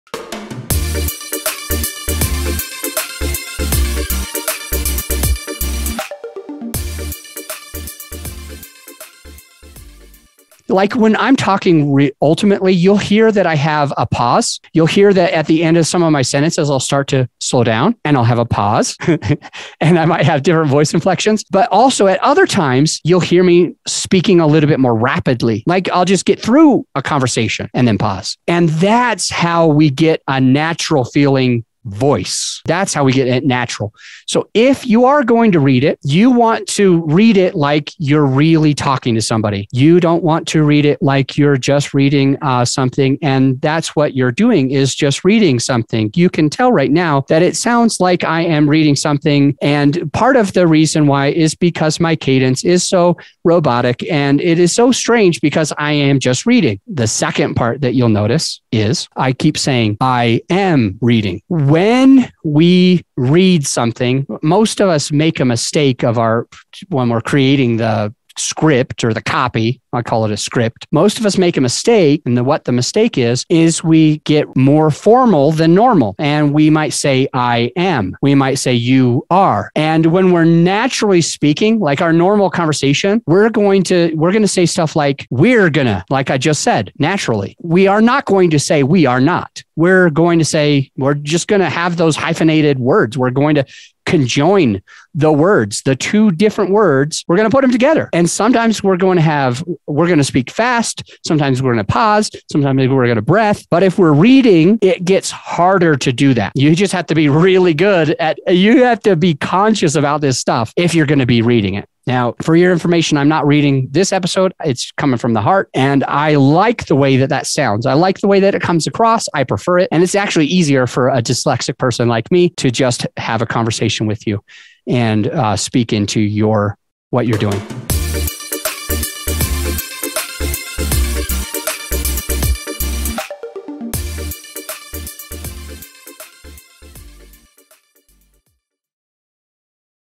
Using Facebook Voice Messages To Grow Your Show
Are you reading some scripts for your show? Check this out as Adams illustrates how to read scripts naturally on your show. Tune in to learn more!